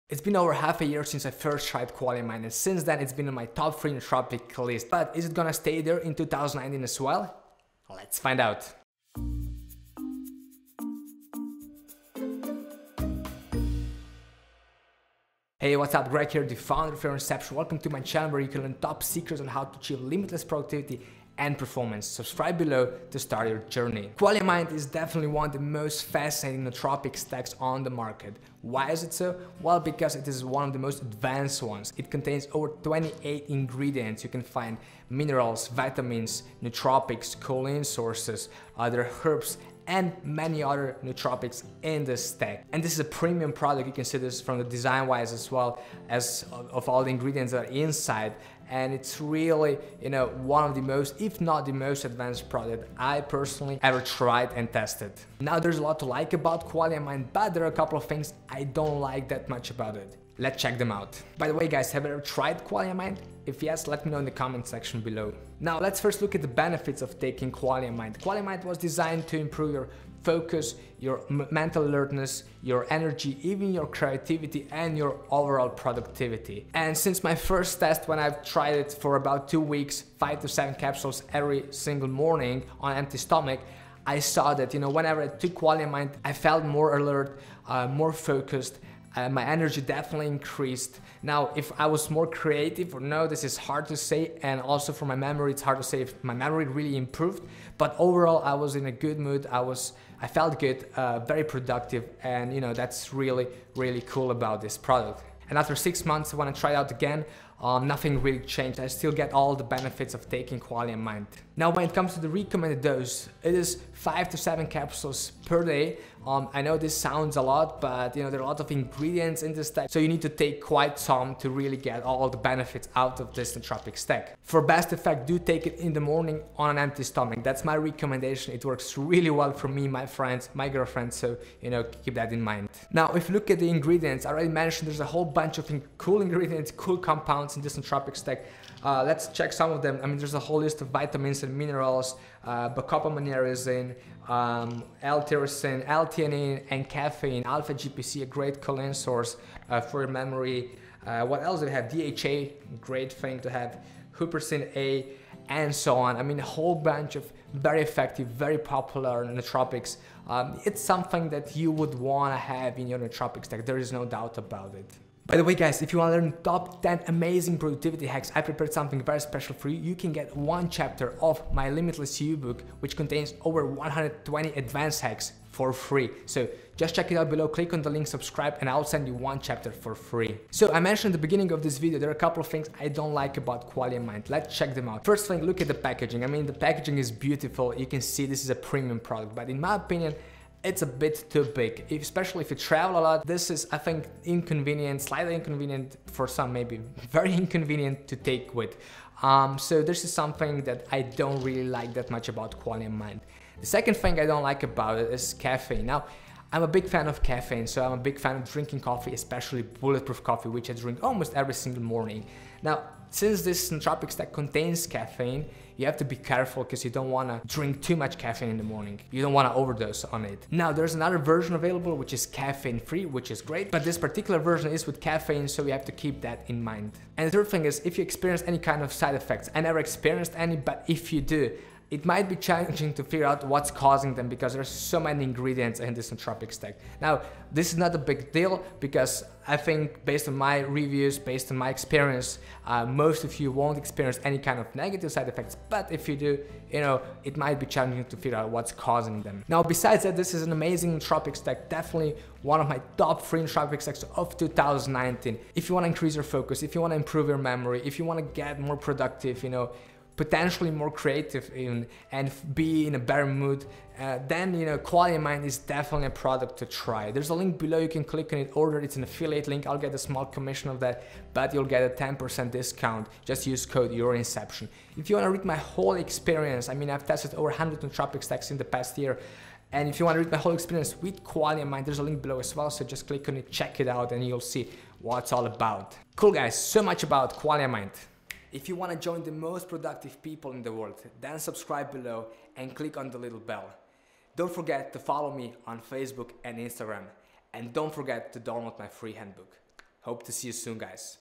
Qualia Mind - After 6 Months Of Taking Qualia Review
Qualia Mind is one of the most complex nootropic supplements on the market. It is also one of the most expensive. But is it worth your money?